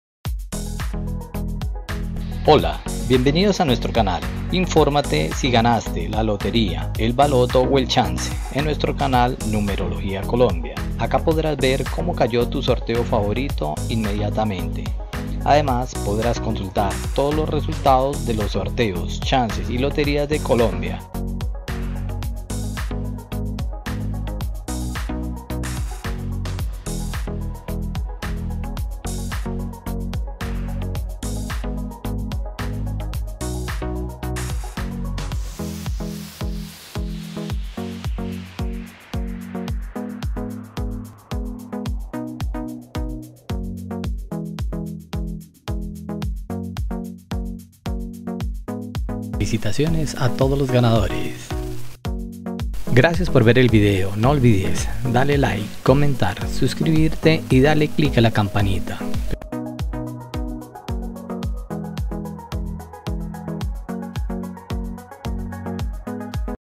Resultado SAMÁN DE LA SUERTE del lunes 27 de septiembre de 2021
Hola buenas tardes, te presentamos los resultados de las loterías y chances en Colombia, vamos con el Resultados del Chance Samán de la suerte del lunes 27 de septiembre de 2021.

loteria saman